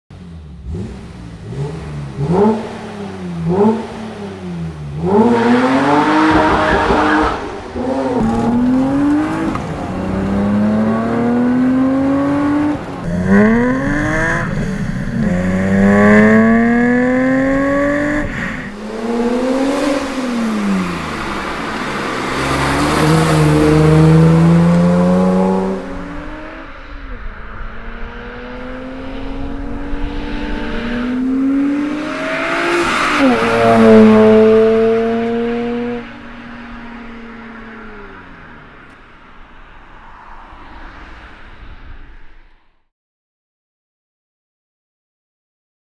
BMW E30 327i Catback uitlaat geluid - HL Motorsport
BMW E30 325e swapped with a 885 cylinder head, inlet, wiring harness and ECU from a 325IX makes the 327i. The HL Motorsport exhaust combined the high revving 2.7l Line 6 makes this BMW sounds amazing.